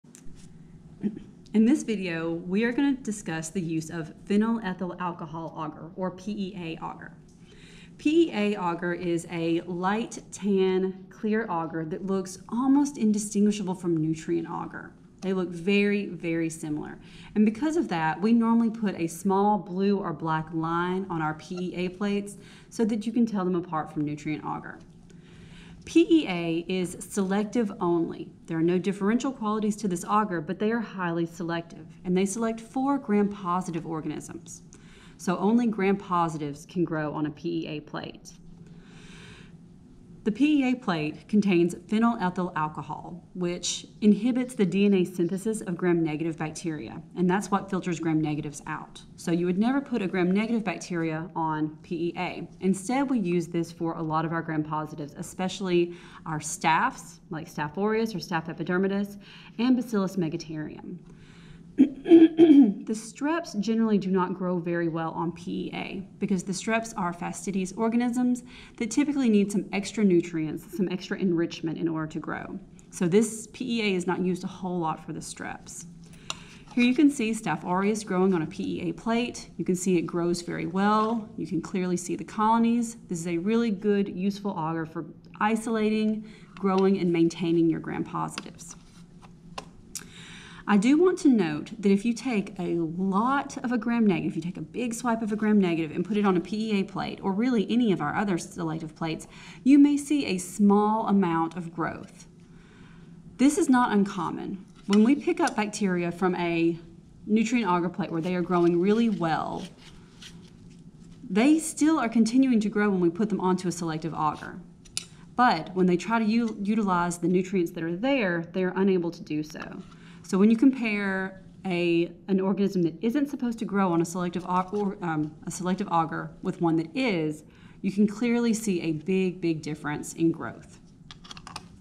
PEA Agar
This video demonstrates what  bacteria grow on this media and how some false readings can occur.